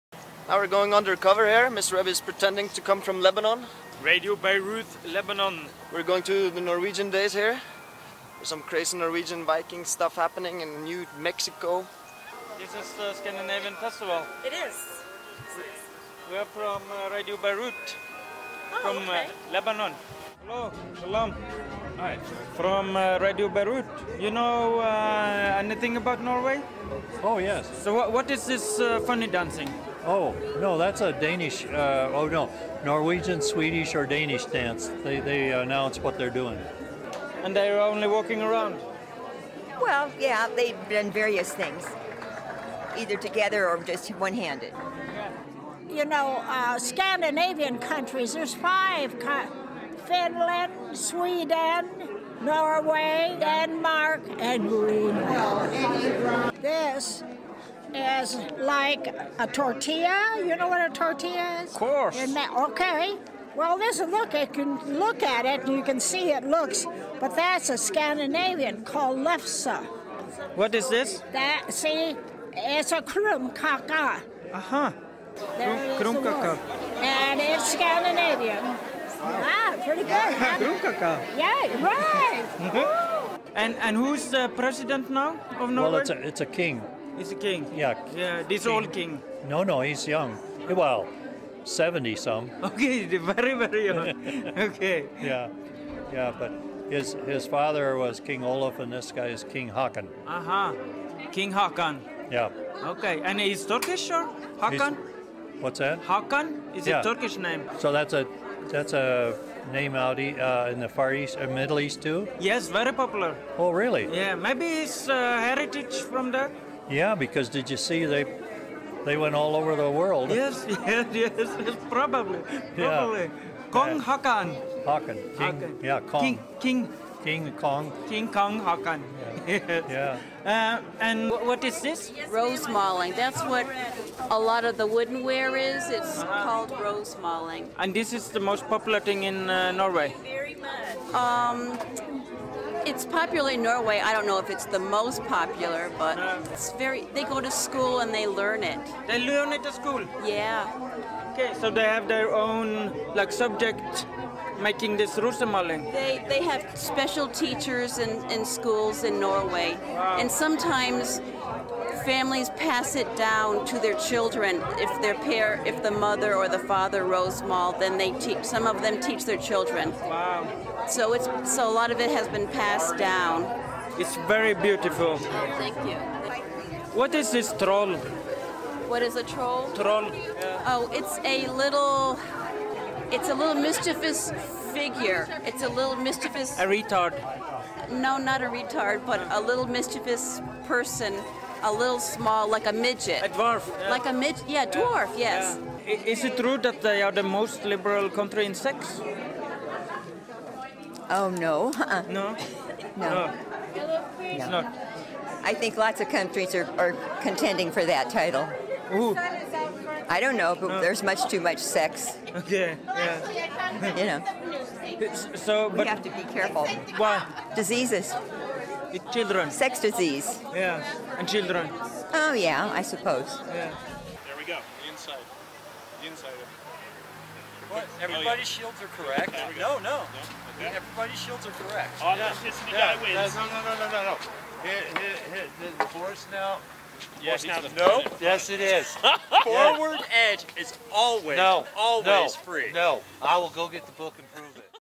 Radio Beirut at a Norwegian Festival in the USA
Disguised as Journalists from the Middle Eastern Country Lebanon, Mr Web and Mr Matias, who are originally from Norway, went to a Norwegian festival in New Mexico to find out how much the Americans knows about Norway.